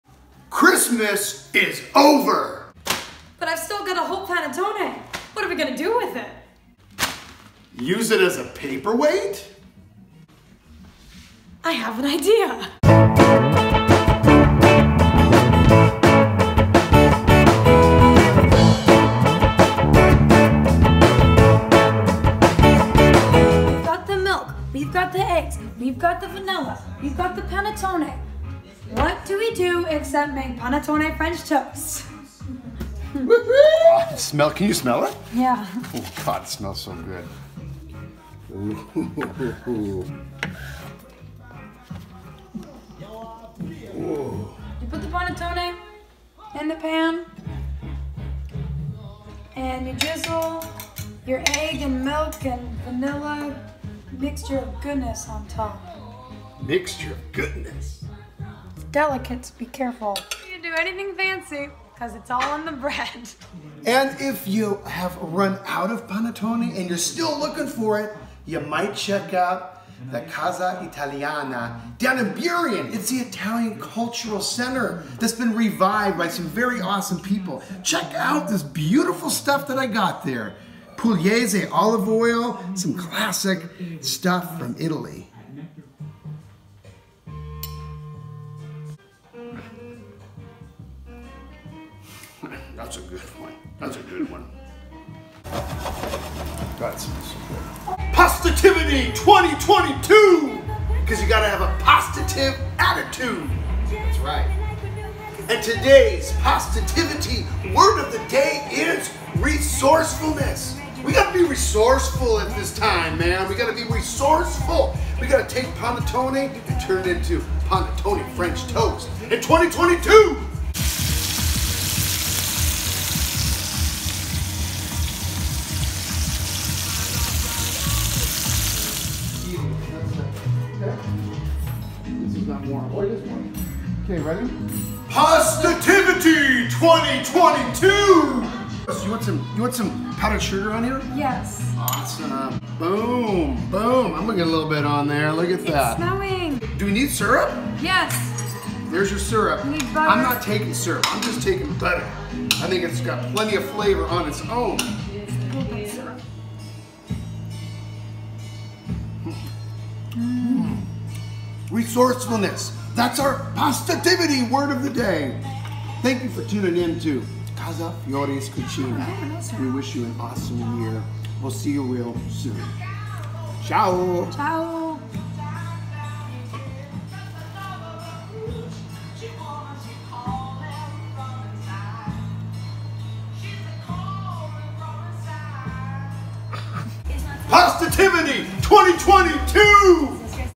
PASTATIVITY 2022! A Fresh Mindset for a New Year! Panettone French Toast
Casa Fiore’s Cucina presents… #62
PASTATIVITY 2022!
A Fresh Mindset for a New Year!
Panettone French Toast